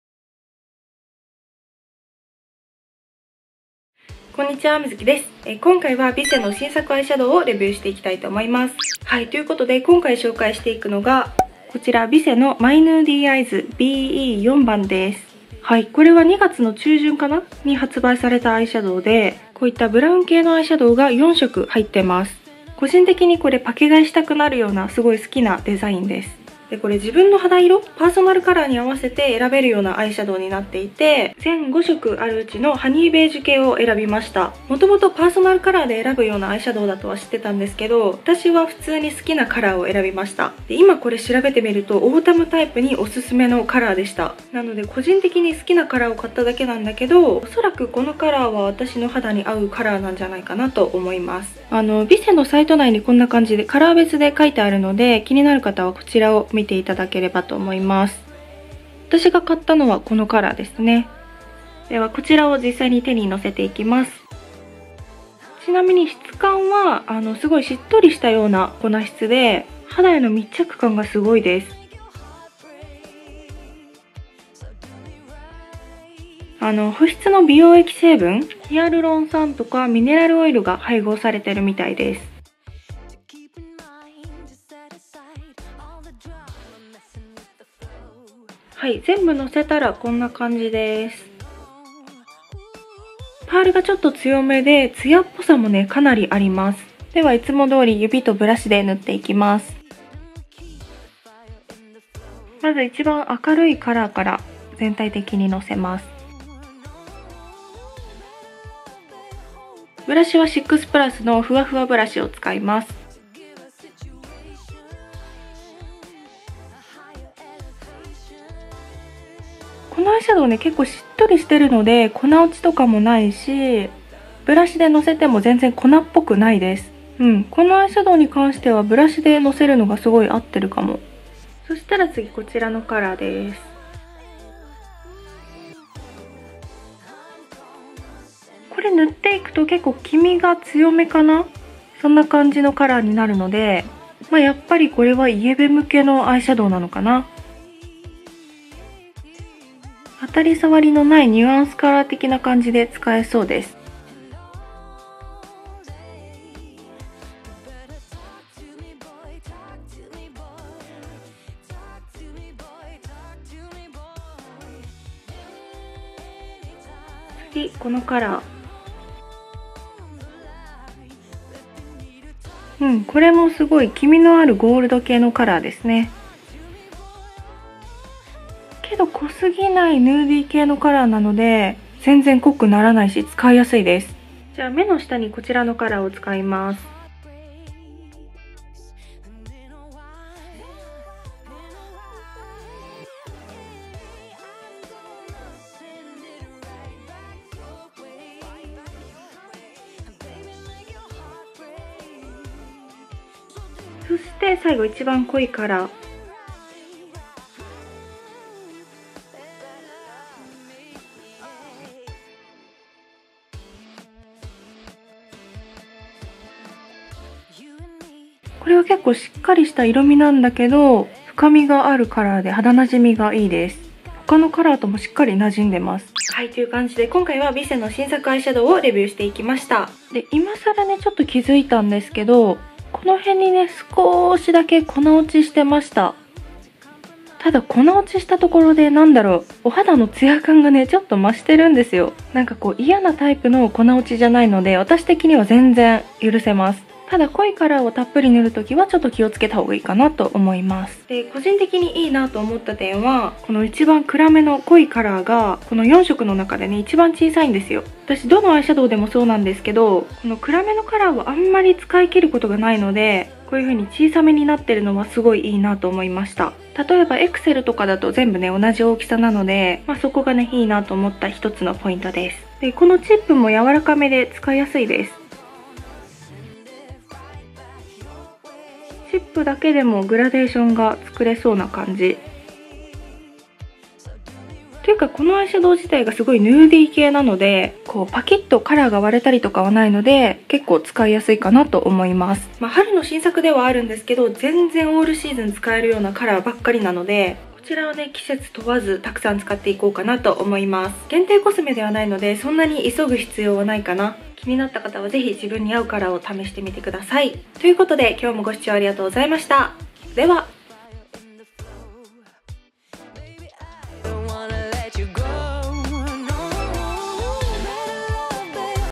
【Visee新作】パーソナルカラー別アイシャドウレビュー❣️オータム向けコスメ【ヴィセ】
久々にVisee（ヴィセ）の新作コスメ買ったのでレビュー！最近こういうパーソナルカラー別のコスメ増えてる気がする、、！結局好きなカラー買っちゃうんだけどね（笑）

ヴィセ リシェ
マイヌーディ アイズ BE-4 ハニーベージュ系

ここにカラー別おすすめアイシャドウが載ってるよ♪

カラコン：THE PIEL モネブラウン

※BGMやサウンドなど動画によって使用してない場合がございます